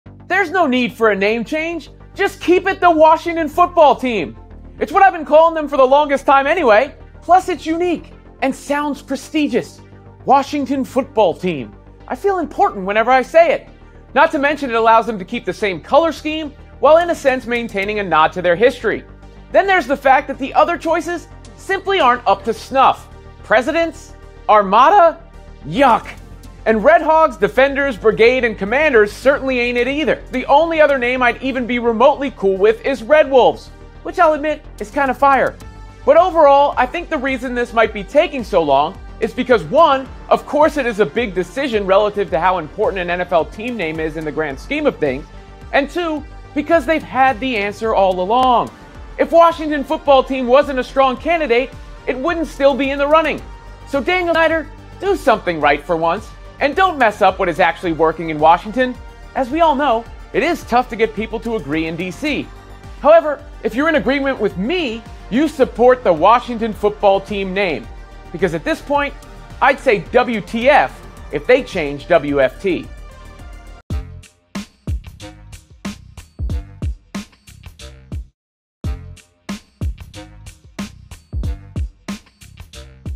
The Washington Football Team Doesn't Need A New Name
The Washington Football Team should just keep the name it already has. The franchise has reportedly narrowed down its options for a new nickname, but rumored choices like the Presidents or Defenders don't compare to the current name that has caught on despite only being meant as a placeholder.

Though we must admit, the Red Wolves does sound pretty cool.
Follow SI on all social channels!